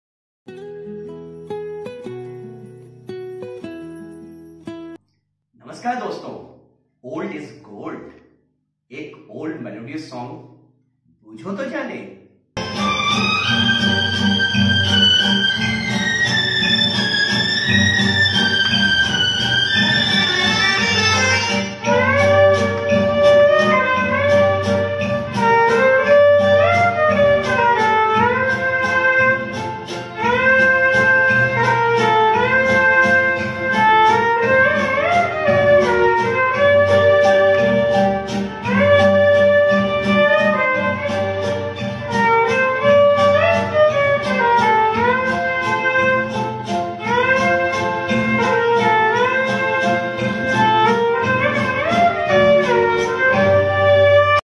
बूझो तो जाने ( एक Musical पहेली ) Ep.9
यदि आप इस गाने को पहचान रहे है तो इस गाने के बारे में और क्या क्या जानते है जरूर comment में लिखे

अपने दोस्तों में भी शेयर कर पूछे कि क्या वो भी ये गाना पहचानते है ?